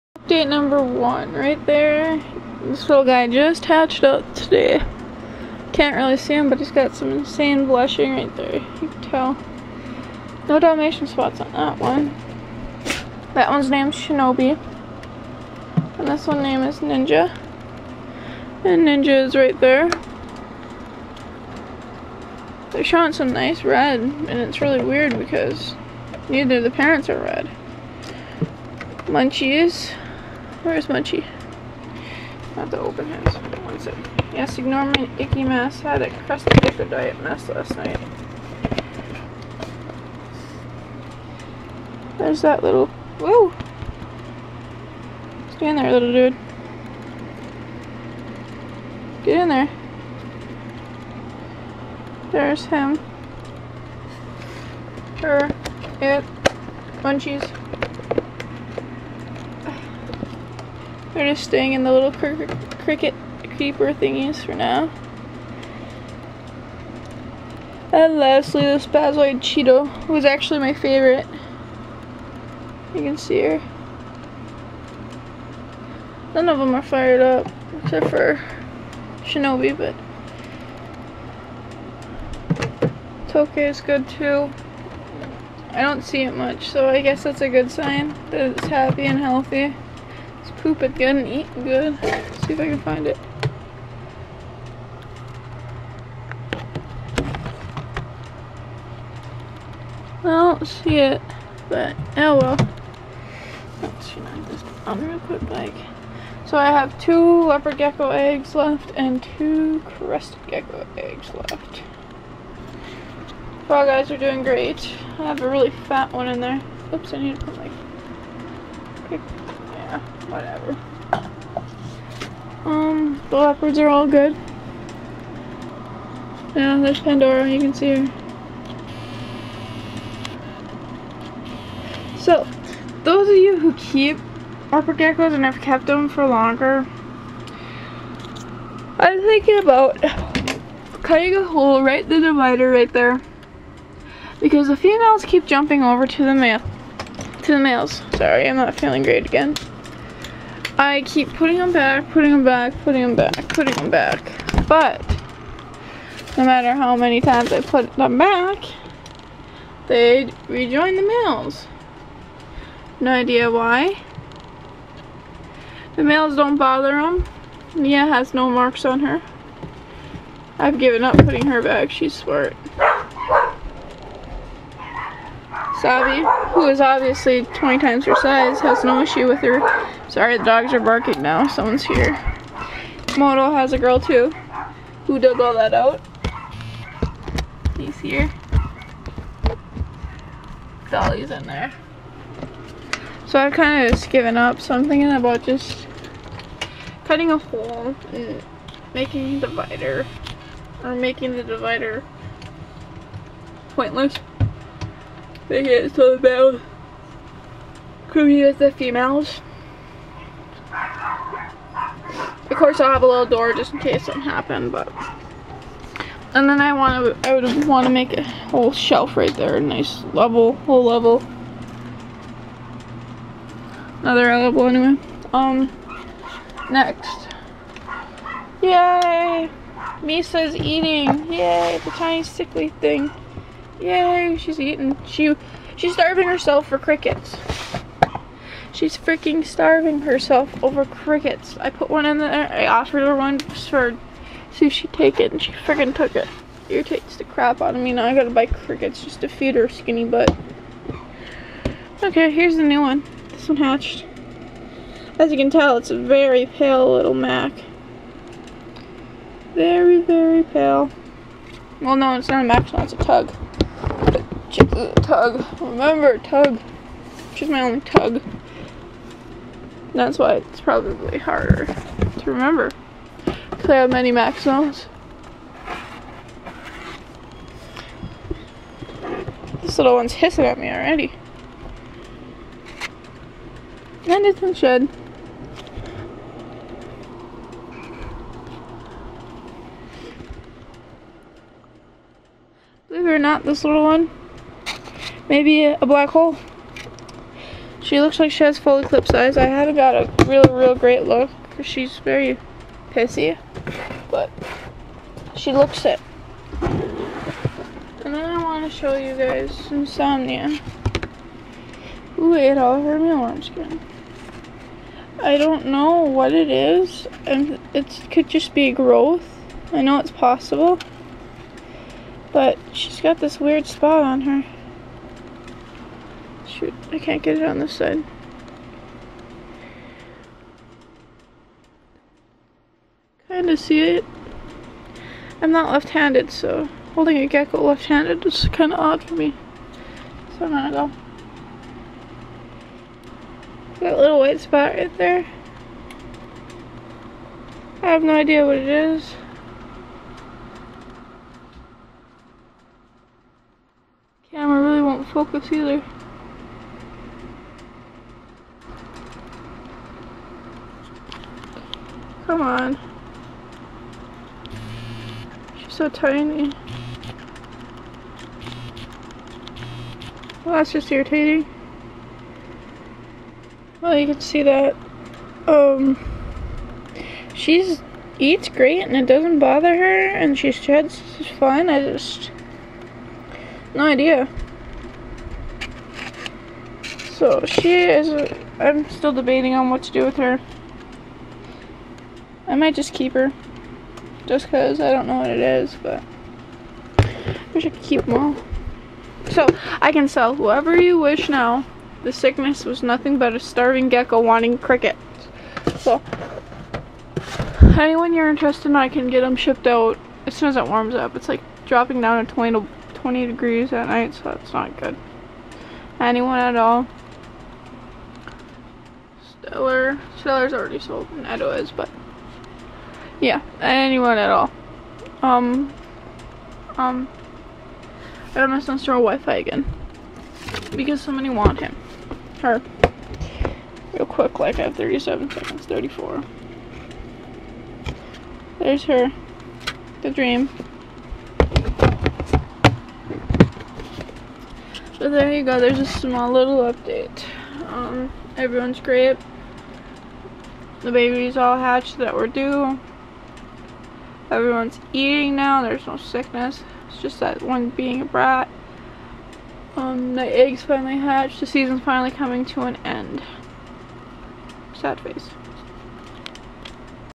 Update!:3 babies hatched and good news
So the one that was sick is eating just fine now! She just wanted crickets! only 2 more eggs of cresties and leos left. Let me know what ya guys think!